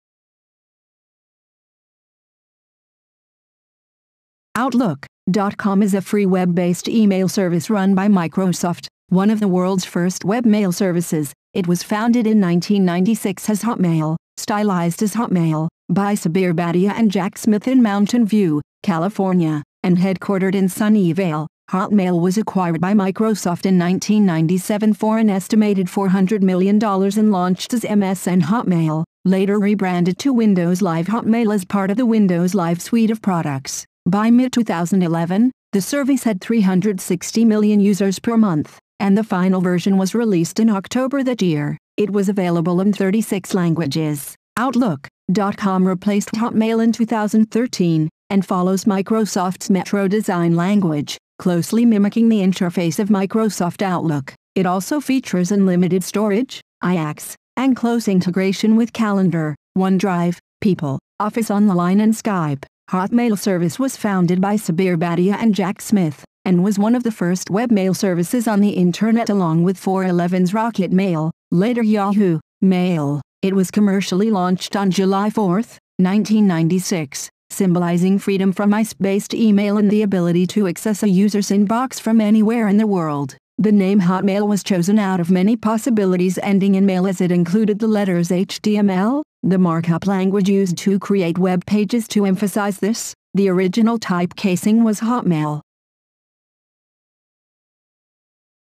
Hotmail - Wiki Videos
Outlook.com is a free web-based email service run by Microsoft. One of the world's first webmail services, it was founded in 1996 as Hotmail (stylized as HoTMaiL) by Sabeer Bhatia and Jack Smith in Mountain View, California, and headquartered in Sunnyvale